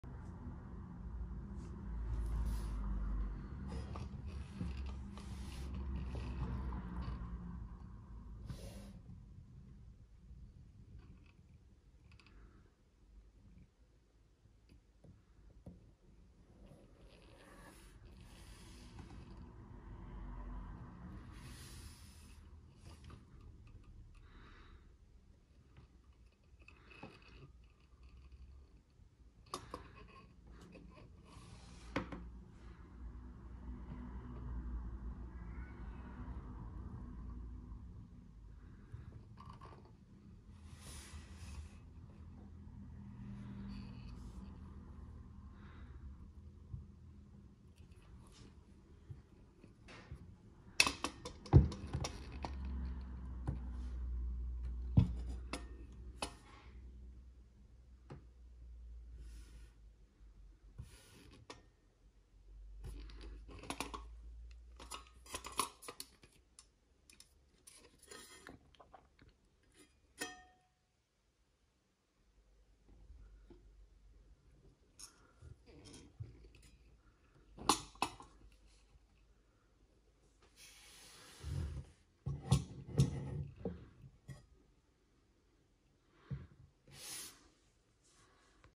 Pedalier Aldhu Carbon aéro 56/44 plateaux ronds NOQ
Un montage idéal pour les vélos de CLM !

Les utilisateurs de capteurs de puissance dans les pédales pourront monter ce pédalier pour avoir le maximum de rendement aéro.

Ceux qui souhaitent le même niveau d'aéro pourront réaliser un montage avec les mêmes plateaux et manivelles, et remplacer l'étoile aéro par le capteur en étoile INspider.